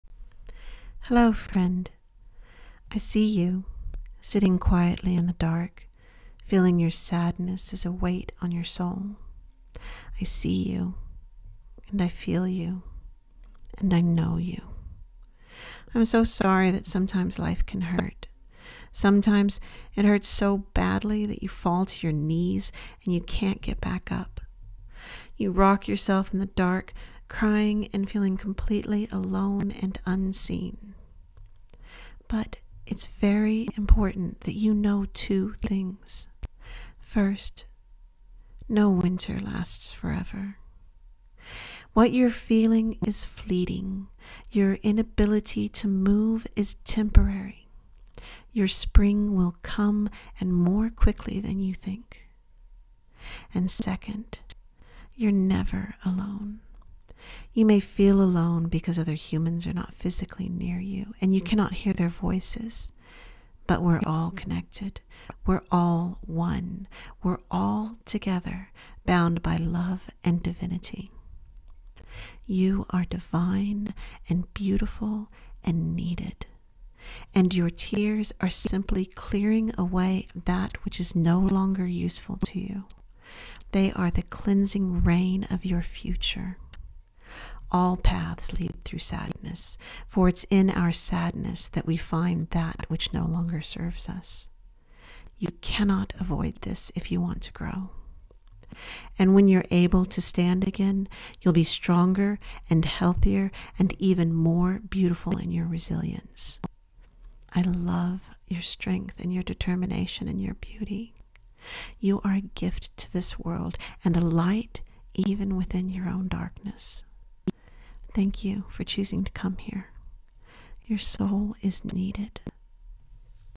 Dear Friend: I feel sad...
I was really sad last week and just couldn't kick it. So, after a couple of days moping around the house, I sat down and wrote a letter like I was writing to a friend who was sad to try to help myself. And I decided to share what I wrote in case it might help anyone else who was having a sad time. Hopefully a little glimmer of hope to help process the sadness.